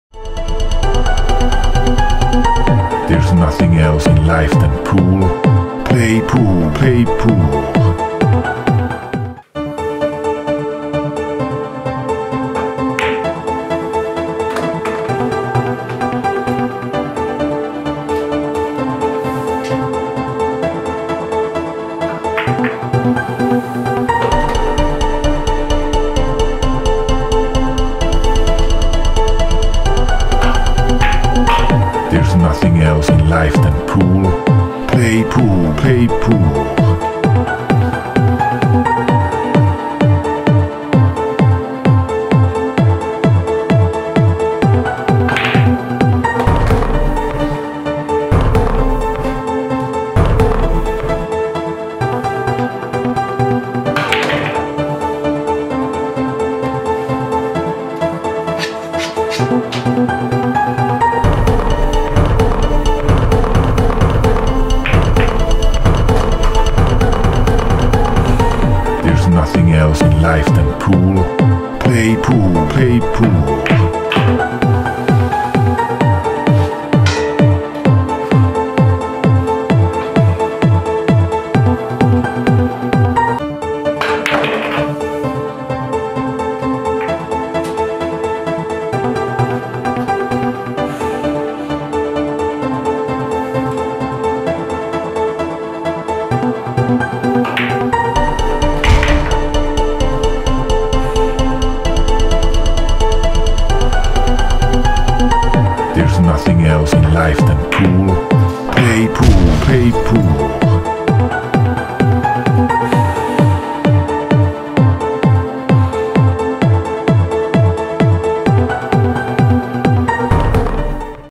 Gas Mask 10ball #18 Extreme inside spin on 9
Table used: Dynamic III, 9ft (tight pockets)
Cue used: Buffalo Century #7 Carom Cue 510g